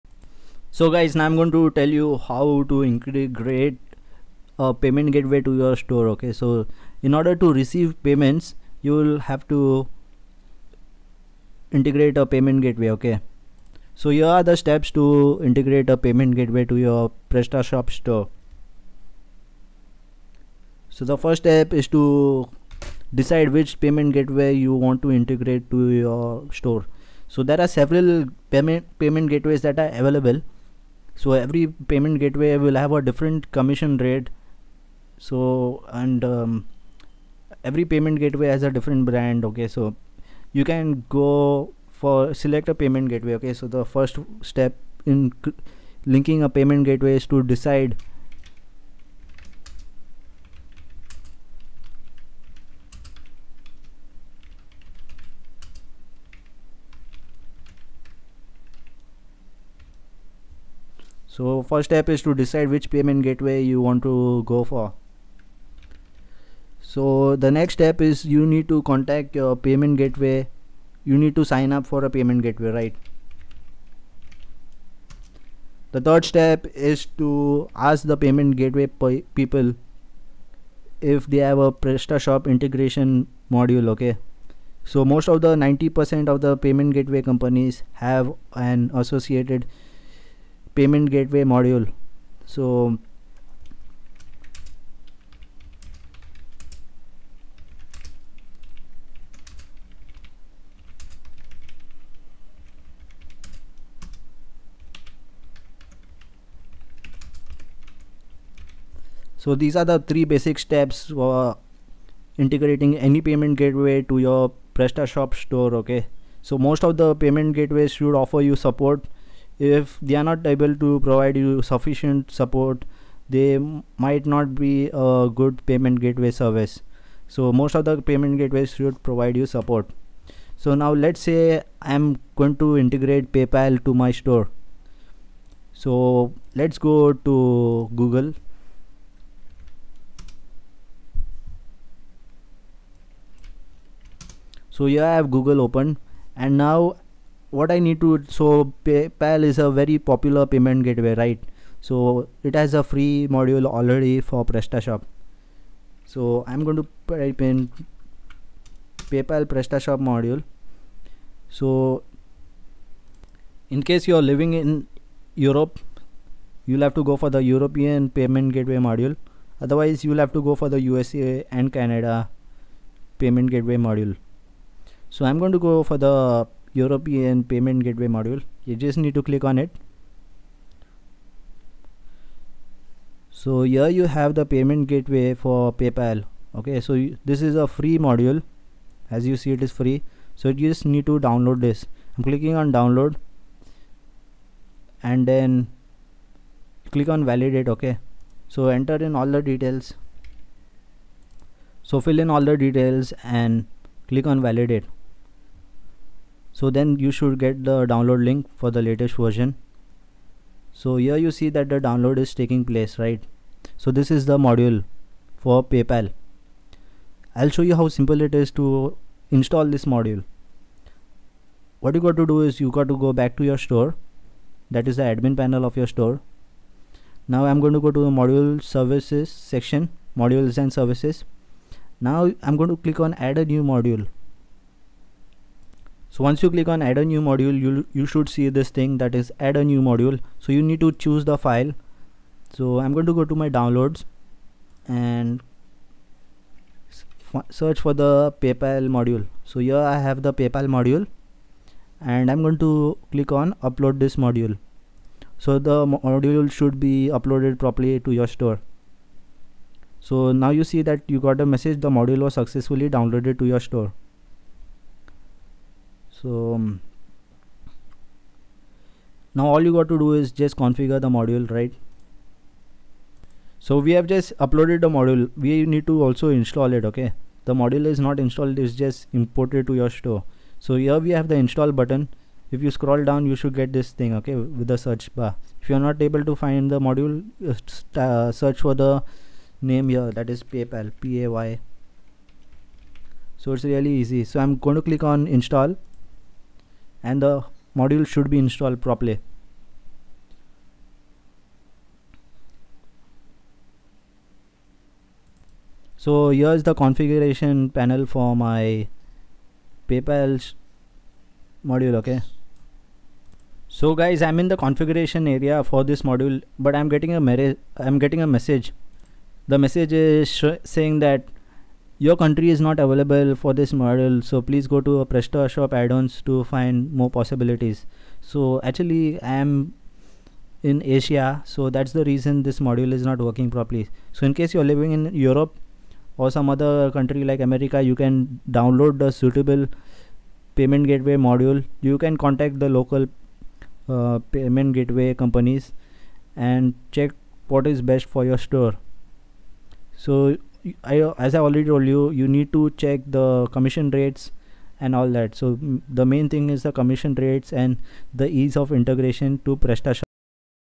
how to Integrate a Payment Gateway to your PrestaShop Store | My Opinions
PrestaShop Webhosting at a Discount

Dear guys in this short Video I provide you with some advice to help you setup a payment gateway for your store.I hope that this advice helps you.

payment gateway integration in prestashop